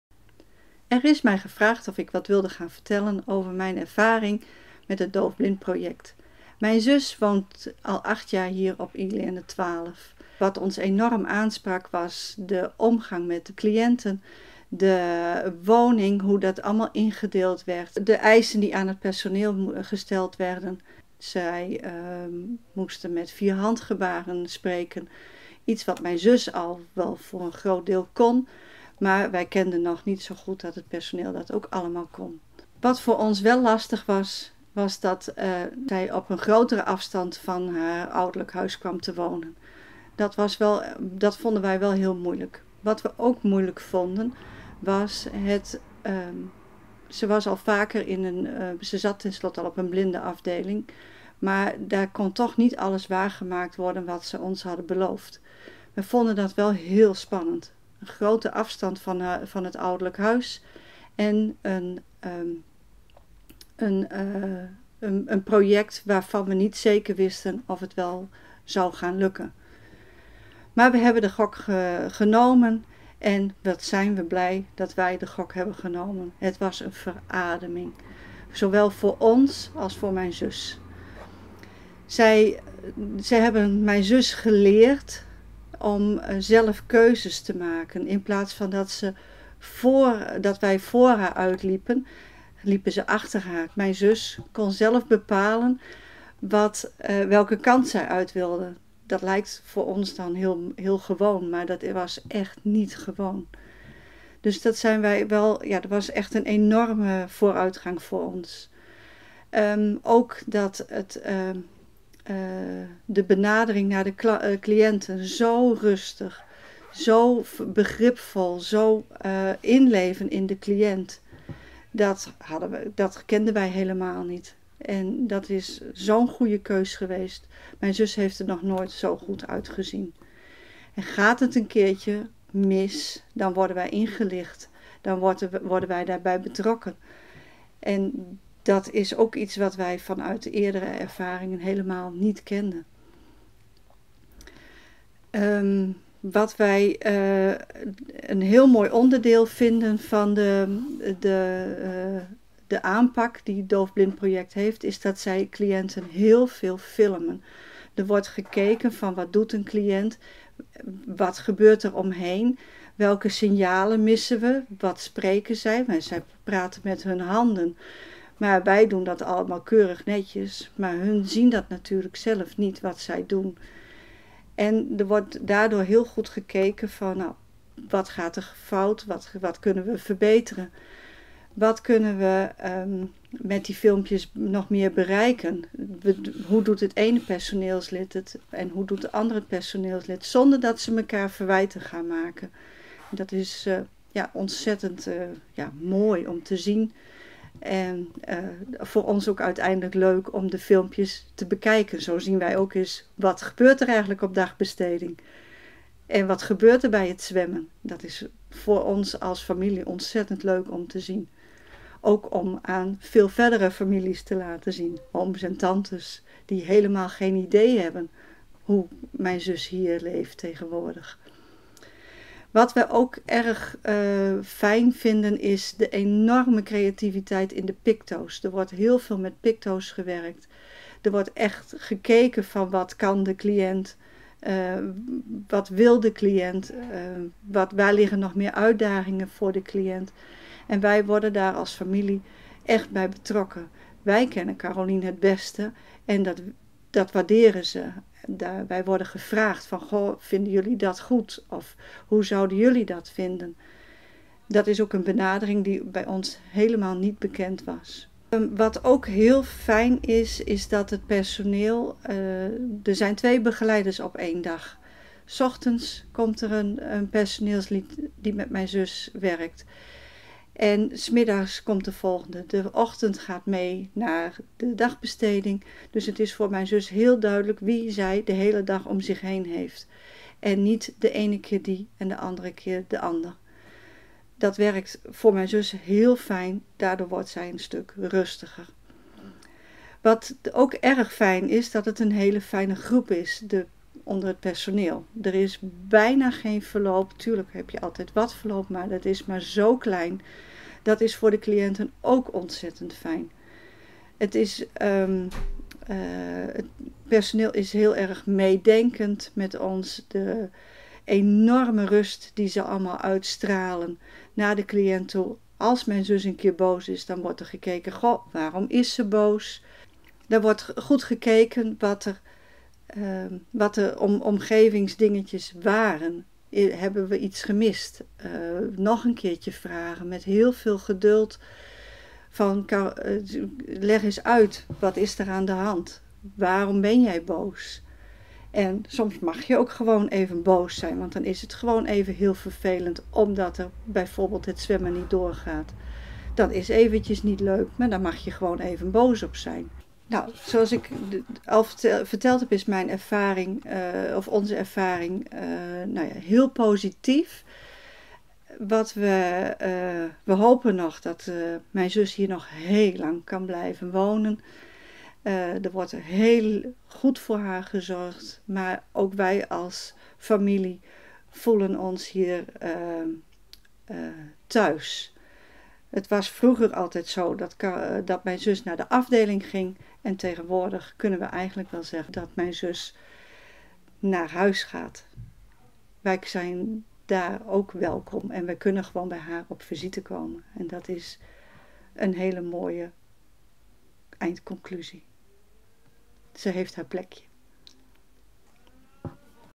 Ervaringsverhaal Charlotte
Charlotte is de zus van een cliënt die op één van onze gehandicaptenzorglocaties in Stiens woont. Op deze locatie wonen cliënten die naast een verstandelijke beperking, een beperking hebben aan het zicht en het gehoor. In deze video deelt Charlotte haar ervaring.